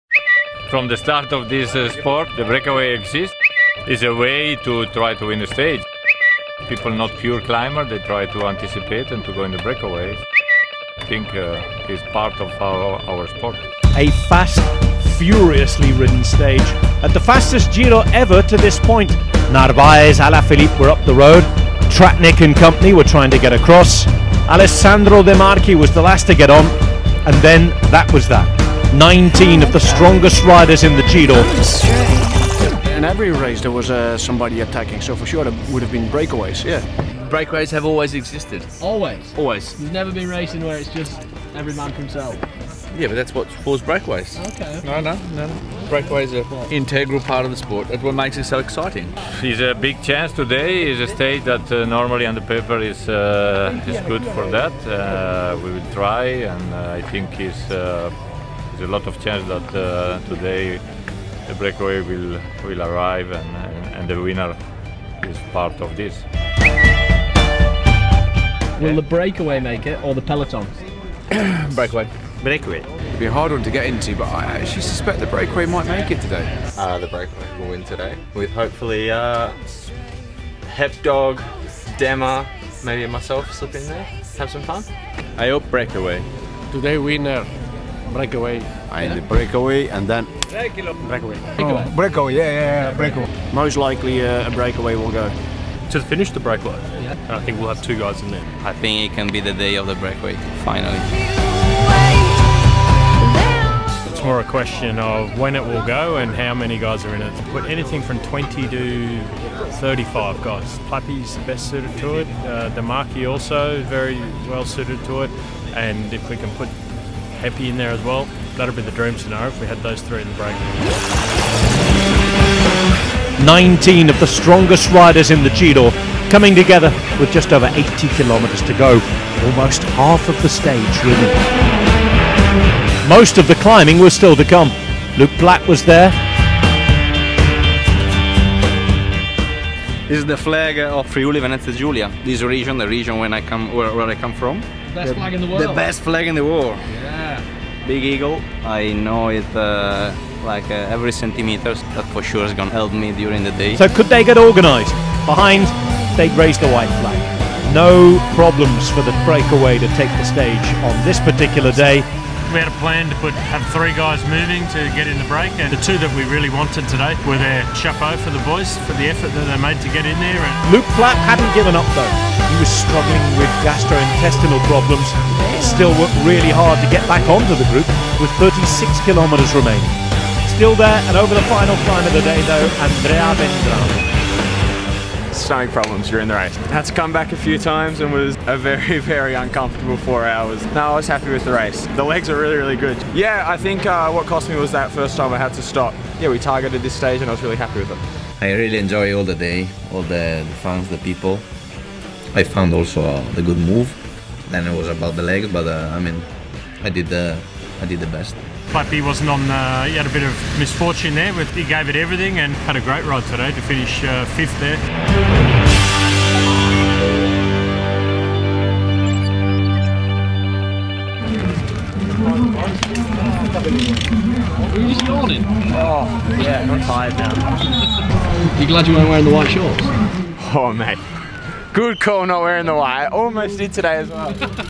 GIRO D'ITALIA 2024 - Stage 19 | Team Jayco AlUla
It’s a breakaway day 📆 DeMa is on home roads 🦅 the boys made the move 👏 Plappy battles stomach problems 💩 but still delivers a strong ride 💪